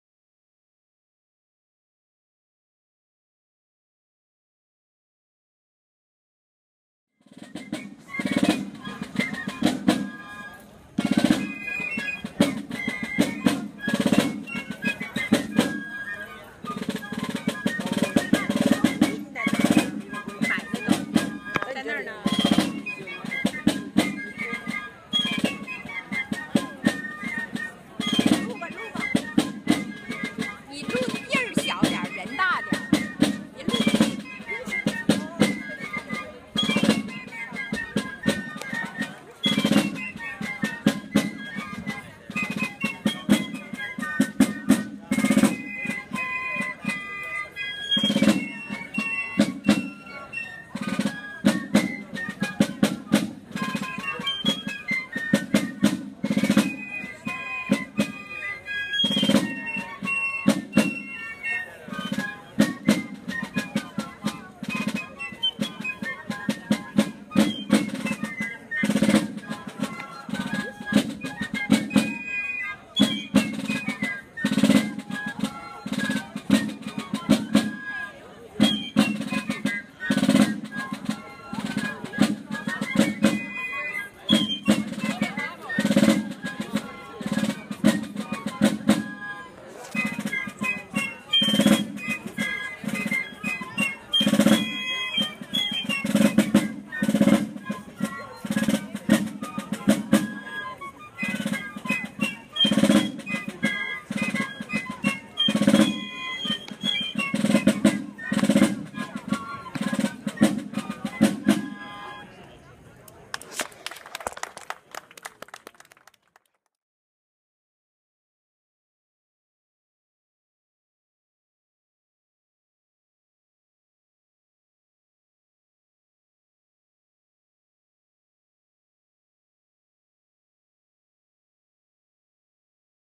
Swiss National Day | Fifes and Drums 1
Video from the Swiss Club of Washington D.C., U.S. on the occasion of the Swiss National Day.